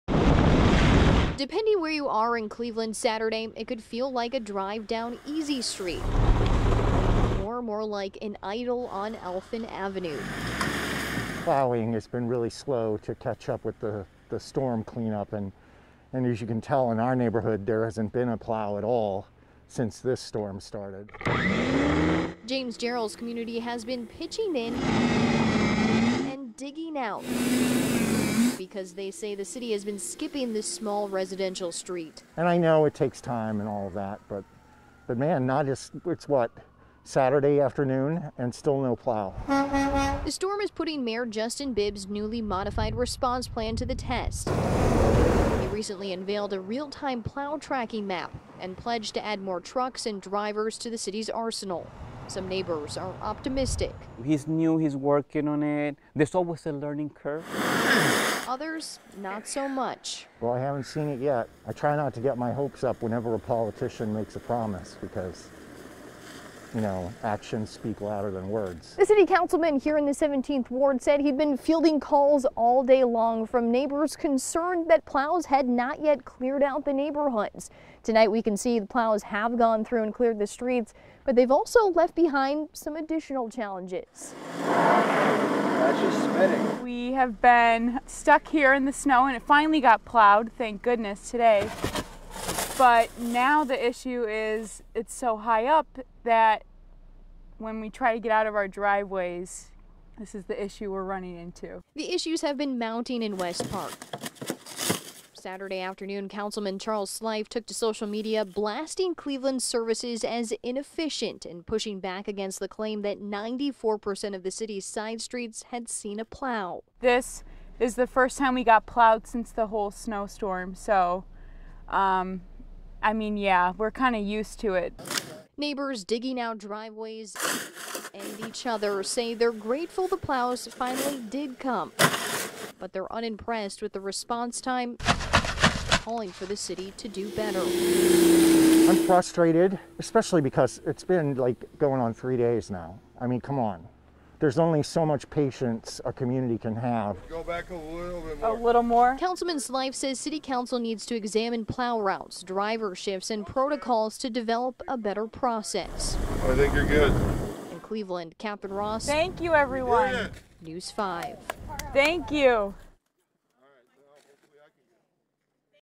Cleveland neighbors frustrated with city's snow response, demand improvements
Parts of Cleveland were still snowed in Saturday afternoon, following the latest round of winter weather. Some neighborhoods waiting for their first pass from plows placed the blame squarely on the city’s response.